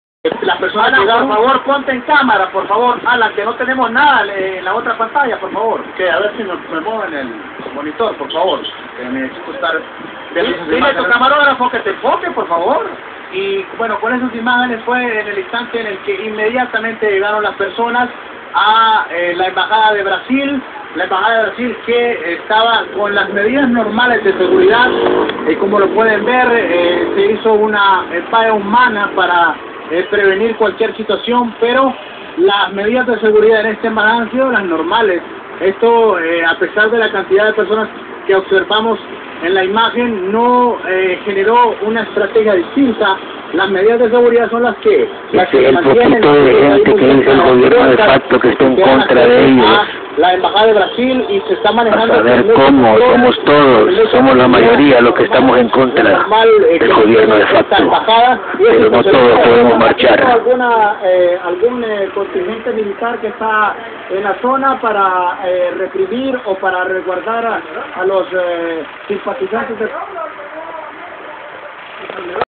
Golpe Estado Honduras, La Resistencia Desafia el Estado de Sitio
Con la llegada del legitimo Presidente de Honduras, el pueblo en Resistencia esta organizandose para marchar hasta la capital y para proteger al Presidente Zelaya, el pueblo esta desafiando el estado de sitio que se implanto el lunes 21 septiembre a partir de las 4:00pm hasta las 7:00pm del martes 22 septiembre